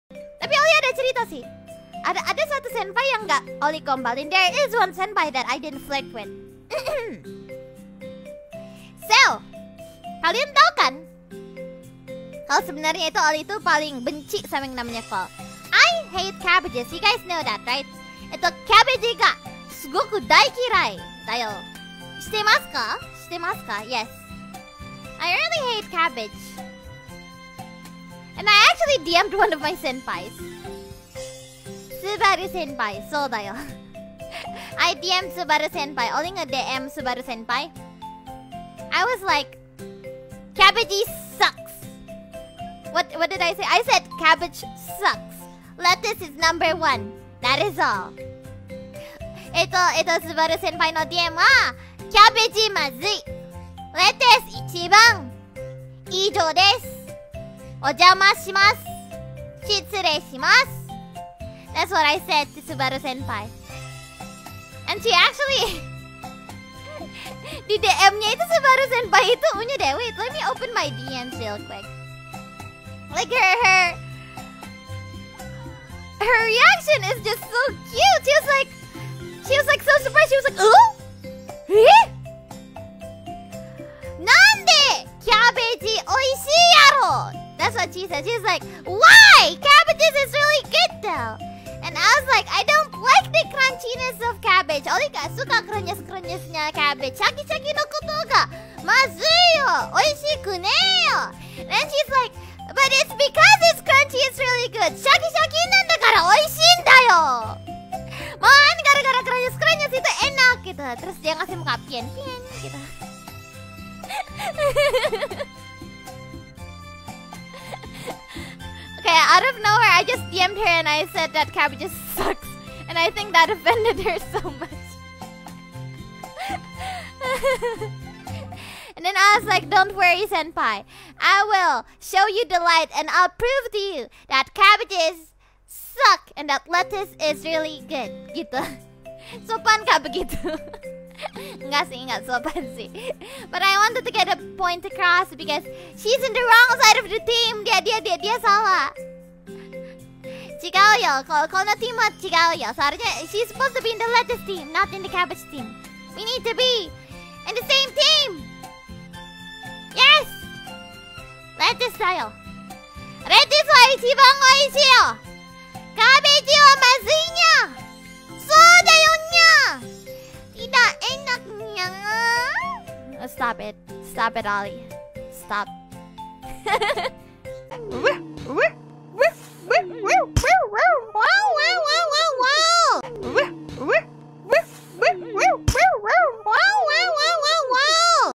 [Kureiji Ollie] The continuation of Subaru's and Ollie's Lettuce Cabbage war
Cabbage or lettuce?

Please support her and watch her stream.